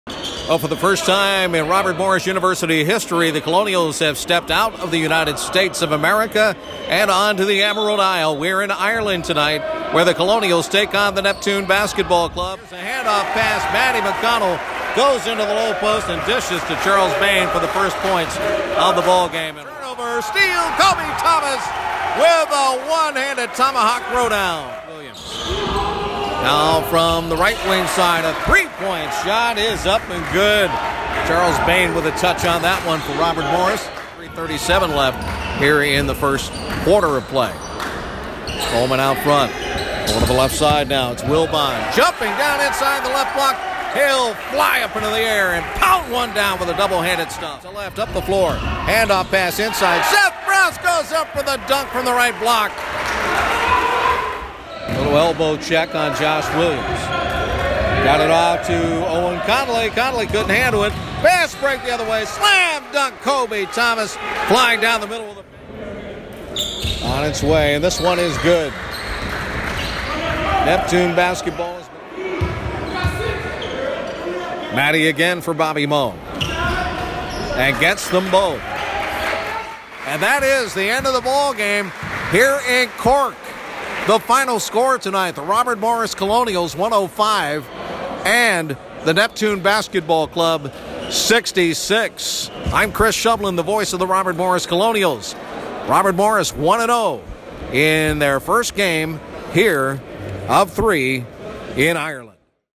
RMU vs Neptune - MBB Highlights
Highlights from the Colonials first game in the Emerald Isle where they defeated Neptune 105-66.